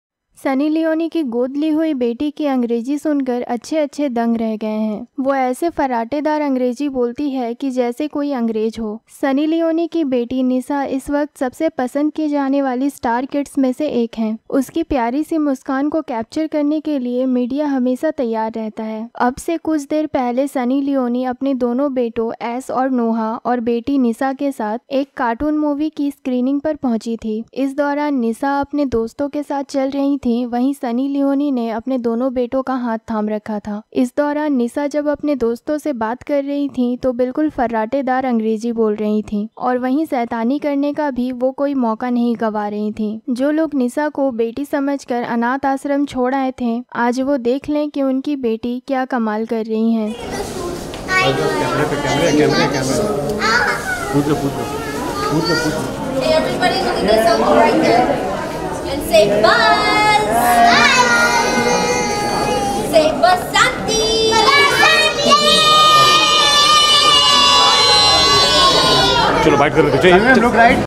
सनी लियॉन की गोद ली हुई बेटी की इंग्लिश सुन कर चकित हो जायेंगे..Sunny Leone's Daughter Spoke English
Sunny Leone's Daughter Nisha Spoke Fluent English In Front Of Media At Screening Of Movie LIGHYEAR